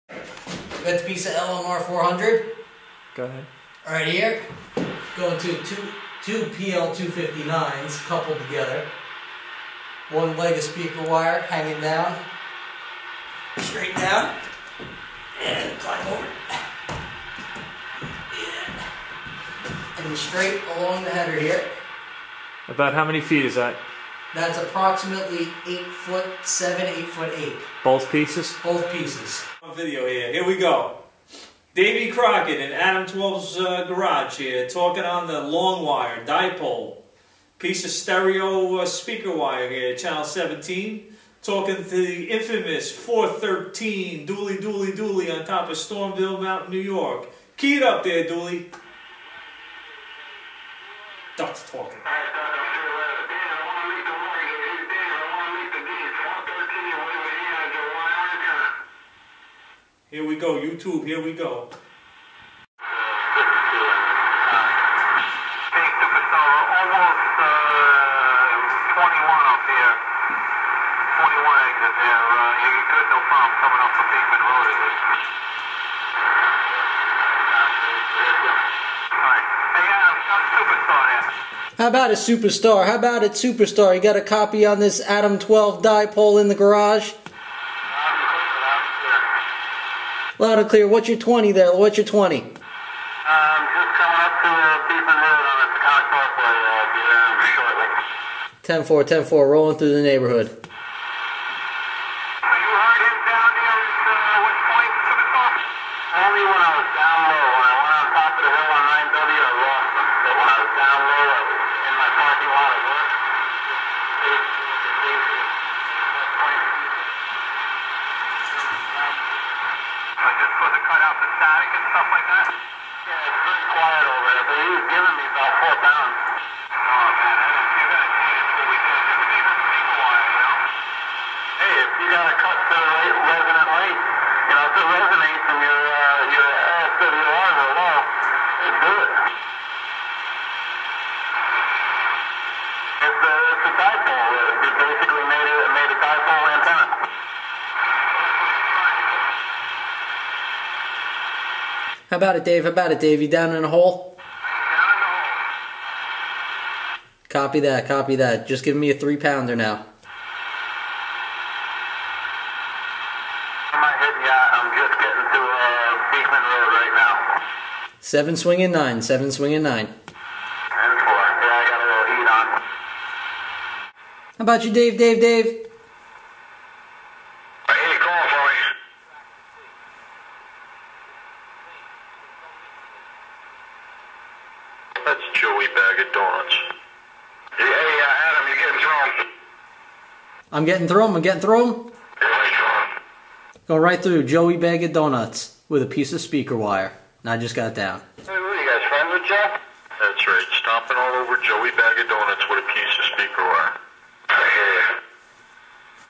Homemade CB Radio Dipole Antenna
Testing a dipole antenna out in the garage.  It's made from some speaker wire and a PL-259.  Video features some prominent Hudson Valley CB'ers.  1-Adam-12 and Davey Crocket (in the garage), Dooley-413, Jack Rabbit and Superstar.  We were using a stock Uniden Washington and a D-104.